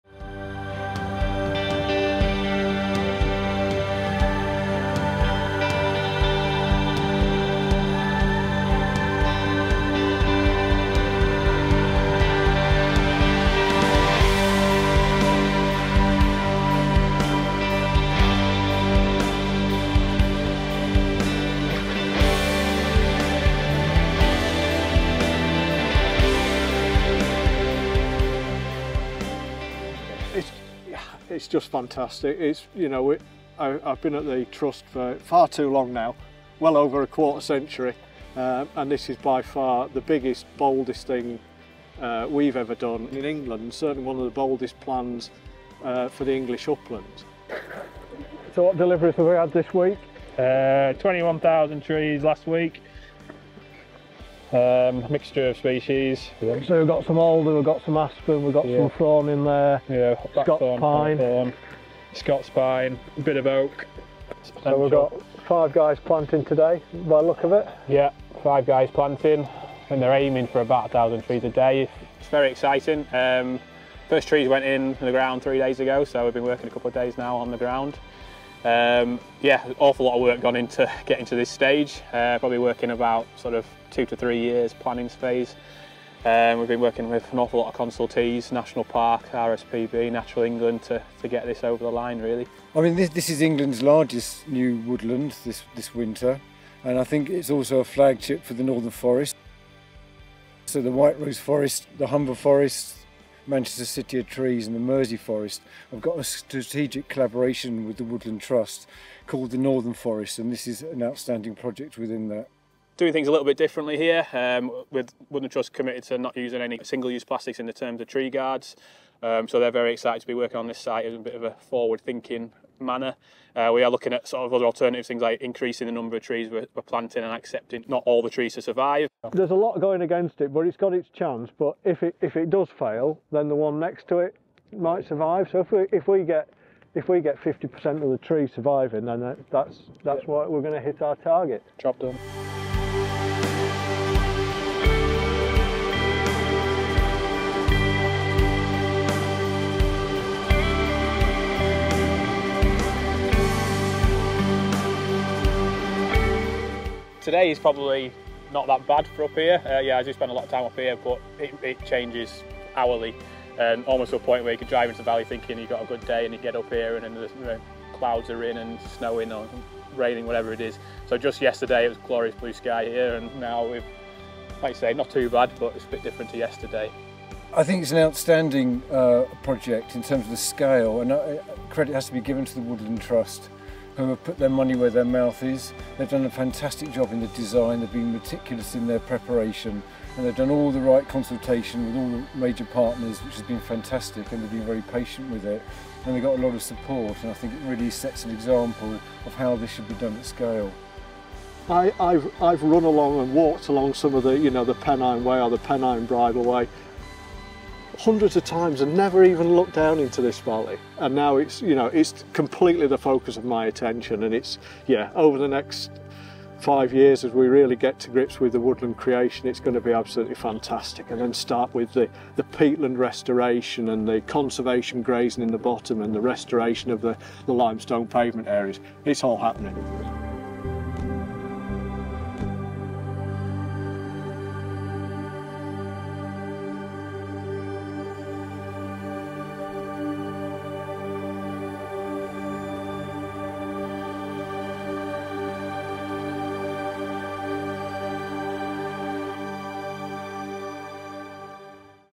Snaizeholme woodland restoration - Tree planting has begun
Thanks to your support, work has now started at Snaizeholme, making it one of the boldest habitat restoration projects in the English uplands.

Snaizeholme is a unique and complex habitat restoration and nature recovery project 20 miles north of Settle, close to the iconic Ribblehead Viaduct in the Yorkshire Dales National Park.

Here, near Hawes, the Woodland Trust is looking to create one of the largest new plastic free native woodlands in England.

Snaizeholme really will be a unique and complex piece of conservation work due the range of habitats and species, the topography and elevation – not to mention the estimated 2m of rainfall per year. Tree planting will exist alongside huge restoration projects, including of 280 acres of upland peatbog, 180 acres of limestone pavement and over 200 acres of riverside meadows following Snaizeholme Beck.